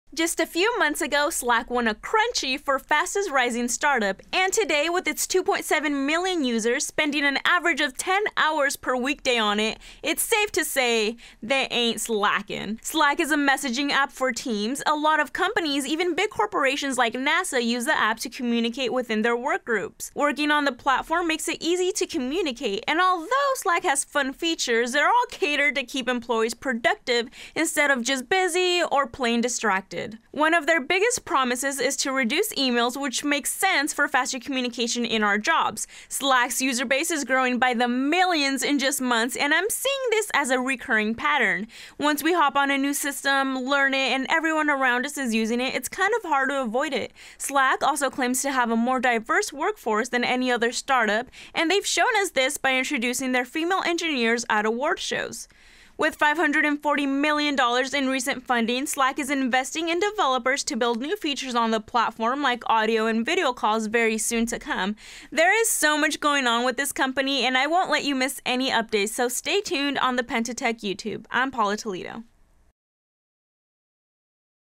Slack App Review
Paola takes a look at Slack, the productivity tool.